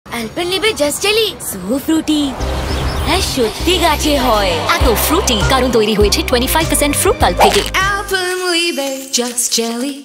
Alpenliebe Juzt Jelly | 2024 Kids and Tree | Bengali (10s)
Discover the secret behind our irresistible jelly – a whopping 25% real fruit pulp that ensures every bite is bursting with fruity goodness!